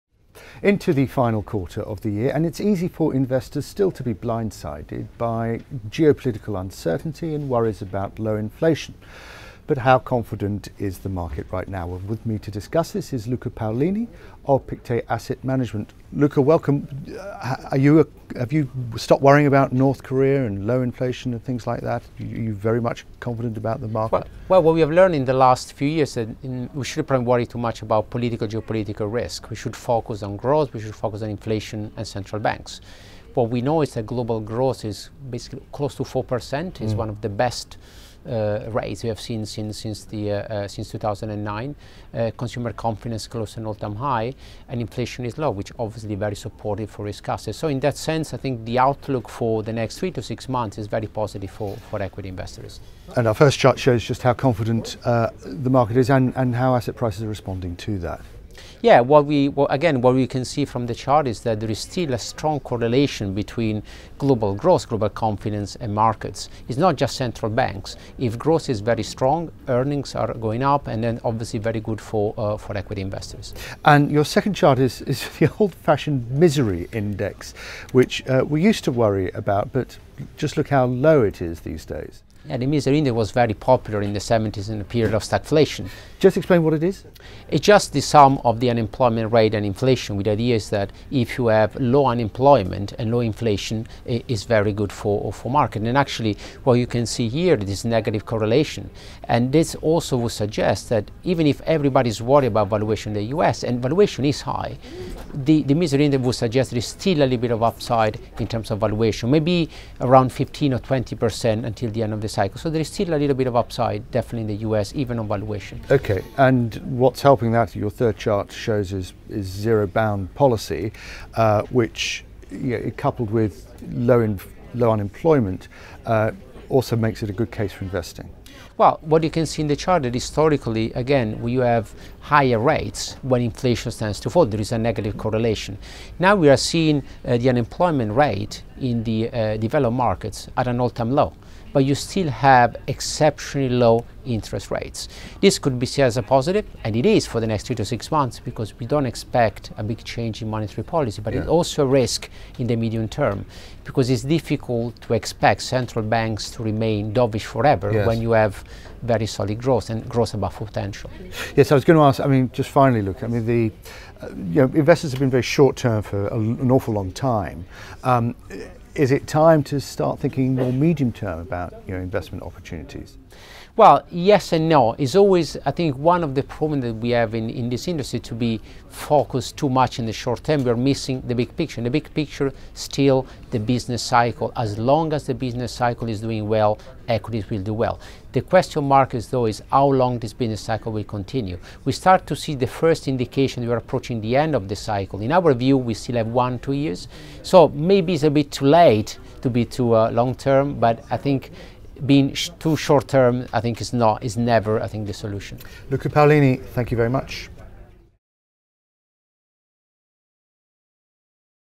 Melt-up before melt-down? | Markets
Roger Blitz, the FT's currencies correspondent, and Luca Paolini, chief strategist at Pictet, discuss how the US 'misery index' (unemployment rate + inflation) has fallen below 6 per cent for the first time since 1998, suggesting we are one to two years away from a major peak